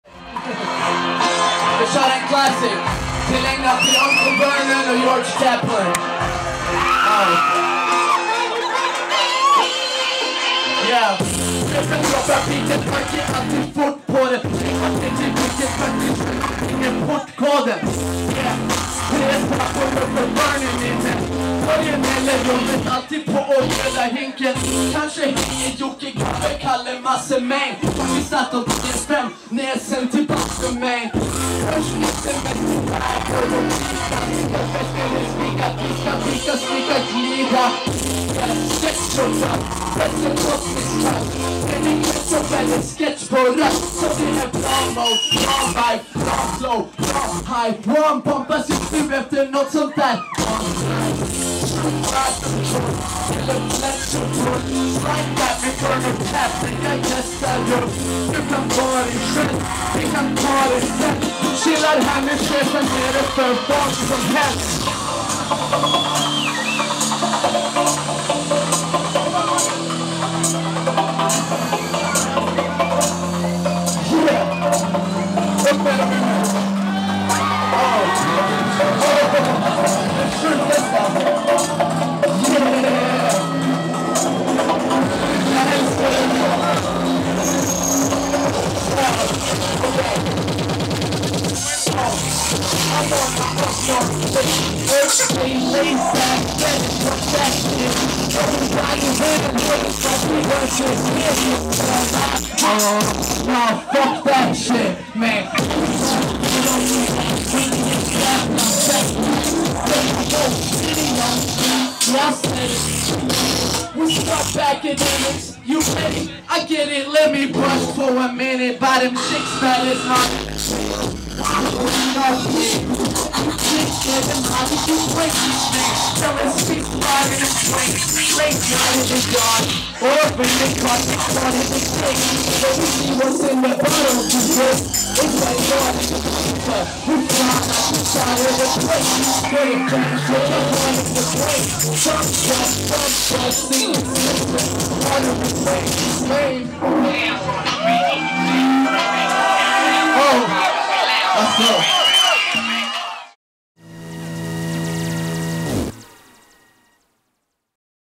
ERK & CHEFEN - Random Bastards Soundsystem live på Goodfellas, UÅ
Vi värmde upp UÅ-publiken innan Flux Pavilion på Goodfellas i januari. Så här såg det ut!